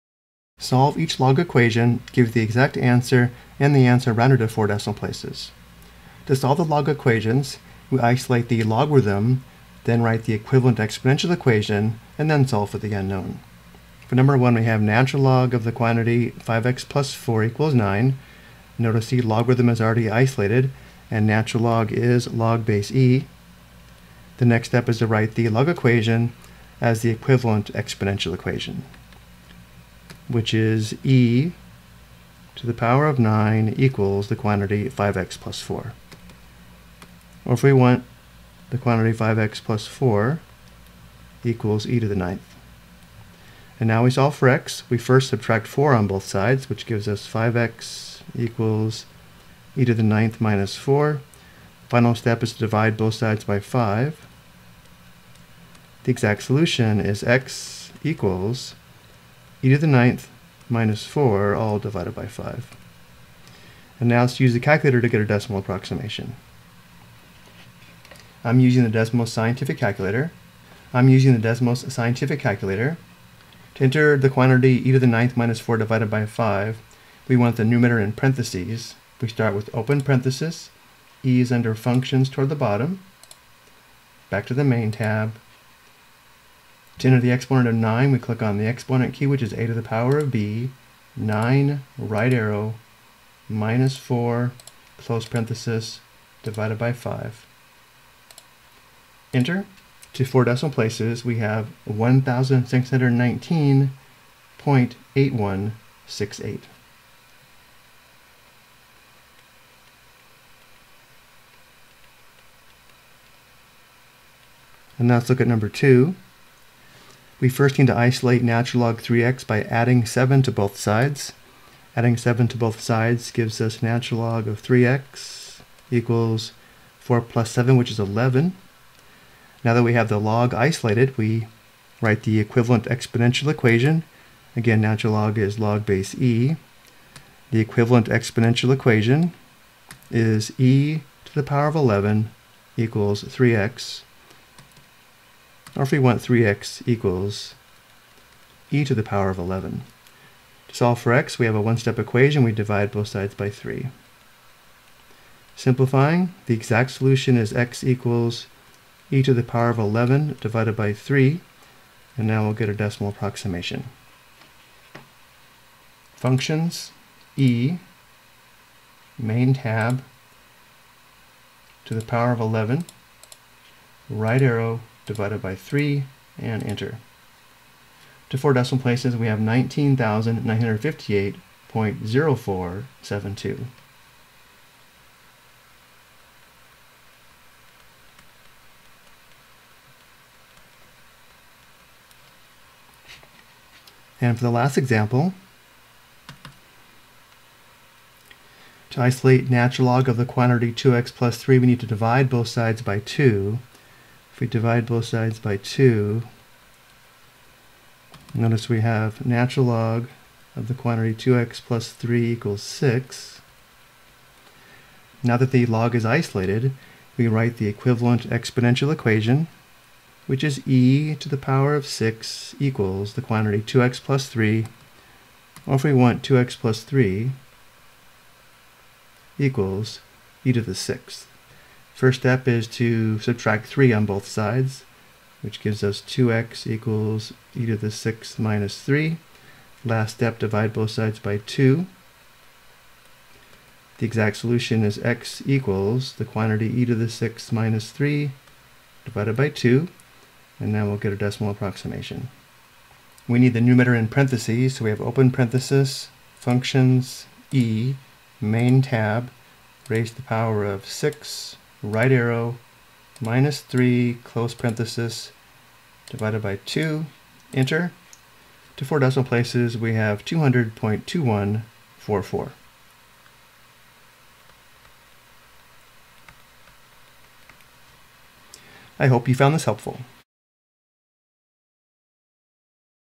Solve Basic Logarithmic Equations With Natural Log
This video explains how to solve basic logarithmic equations with natural log.